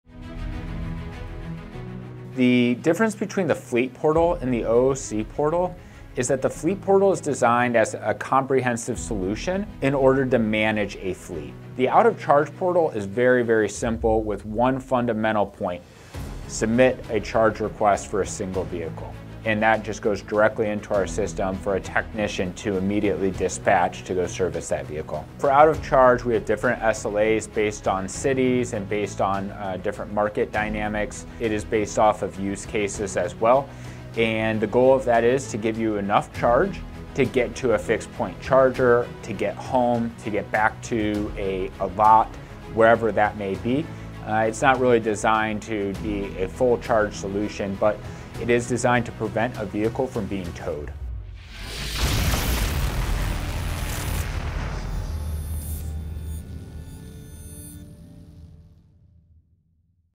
What is Out of Charge?
Out of Charge is a charging-as-a-service (CaaS) offering by SparkCharge that brings the EV charge to the vehicle when it is low on charge. Depending on what is needed, our delivery technicians will deliver between 25-50 miles of range so the vehicle can make it to its' destination. This saves the EV owner the hassle, time, and money of getting a flatbed tow truck.